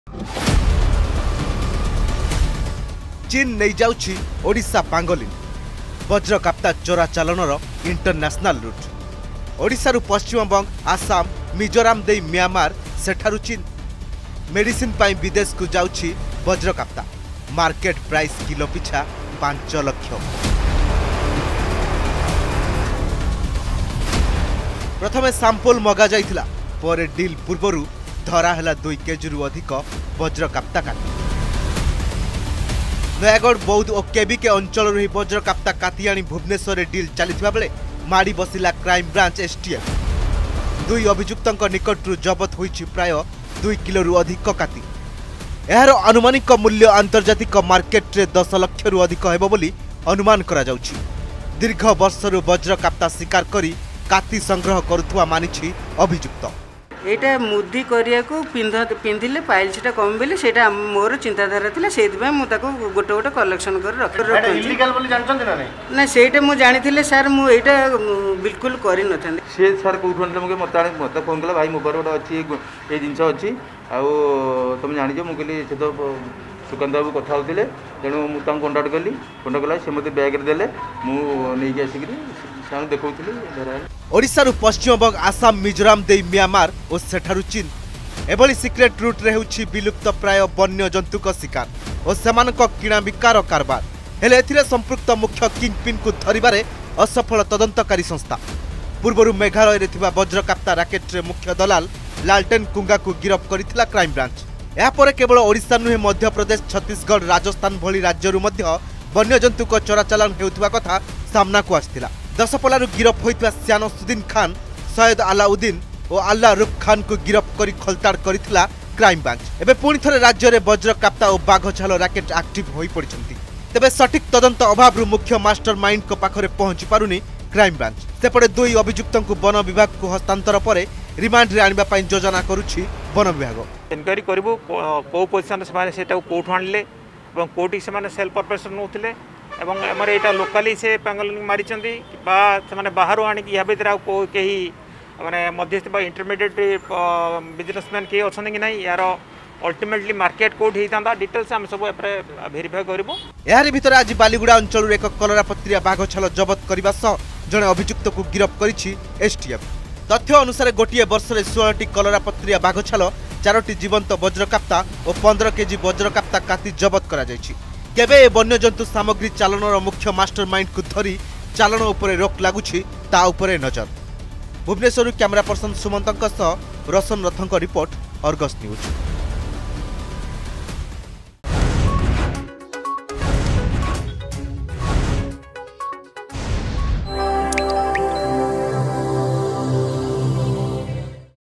ଚୀନ ନେଇଯାଉଛି ଓଡ଼ିଶା ପାଙ୍ଗୋଲିନ୍
ମେଡ଼ିସିନ ପାଇଁ ବିଦେଶକୁ ଯାଉଛି ବଜ୍ରକାପ୍ତା I ମାର୍କେଟ ପ୍ରାଇସ କିଲୋ ପିଛା 5 ଲକ୍ଷ I  ଏଭଳି ସିକ୍ରେଟ ରୁଟରେ ହେଉଛି ବିଲୁପ୍ତ ପ୍ରାୟ ବନ୍ୟ ଜନ୍ତୁଙ୍କ ଶୀକାର ଓ ସେମାନଙ୍କ କିଣାବିକାର କାରବାର ।